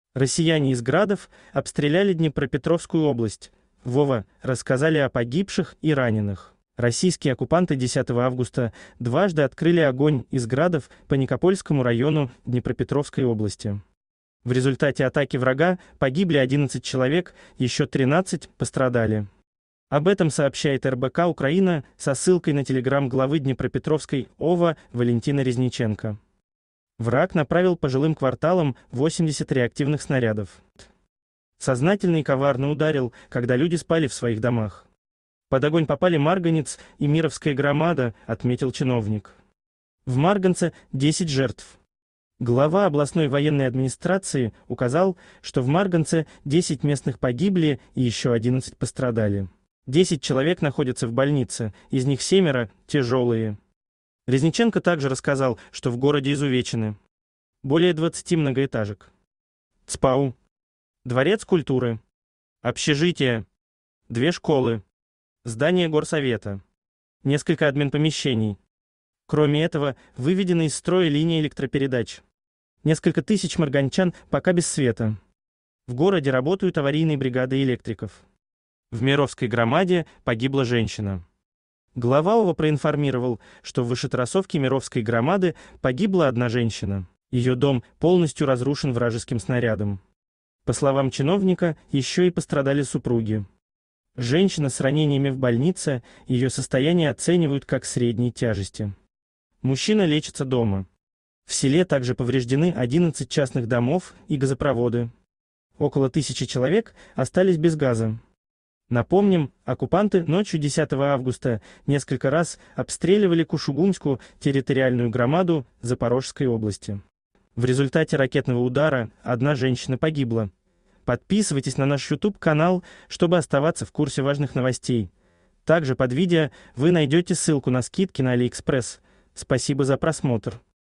Россияне из "Градов" обстреляли Днепропетровскую область: в ОВА рассказали о погибших и раненых.
Днепропетровскаяобласть ВойнаРоссиипротивУкраины Получи скидку на Алиэкспресс по этой ссылке до 70% + дополнительные скидочные купоны от продавцов. Российские оккупанты 10 августа дважды открыли огонь из "Градов" по Никопольскому району Днепропетровской области. В результате атаки врага погибли 11 человек, еще 13 - пострадали. Об этом сообщает РБК-Украина со ссылкой на Telegram главы Днепропетровской ОВА Валентина Резниченко. "Враг направил по жилым кварталам 80 реактивных снарядов. Сознательно и коварно ударил, когда люди спали в своих домах. Под огонь попали Марганец и Мировская громада", - отметил чиновник. В Марганце-10 жертв.
Глава областной военной администрации указал, что в Марганце 10 местных погибли и еще 11 пострадали.  Десять человек находятся в больнице, из них семеро - тяжелые. Резниченко также рассказал, что в городе изувечены